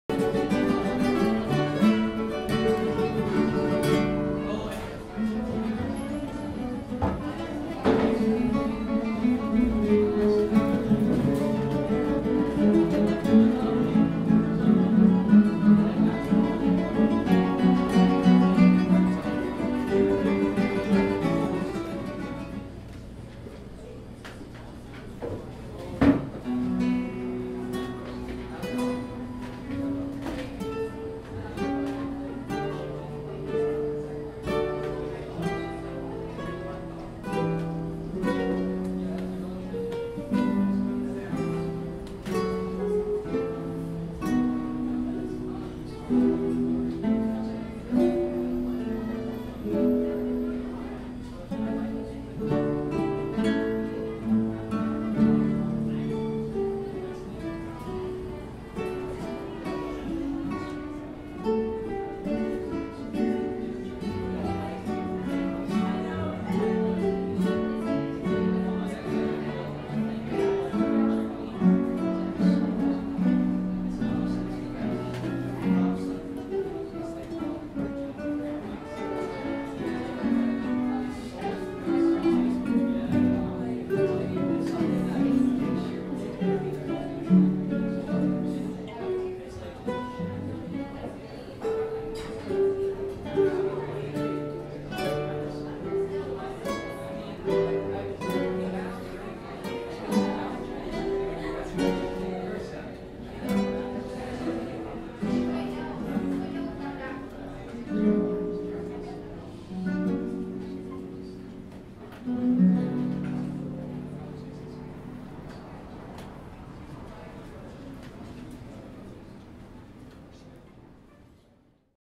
SOTA @ Sweet Inspirations Cafe November 14th, 2011 pt2
Students from the SOTA orchestra perform in ensemble groups at Sweet Inspirations Cafe on Market St in SF on monday November 14th, 2011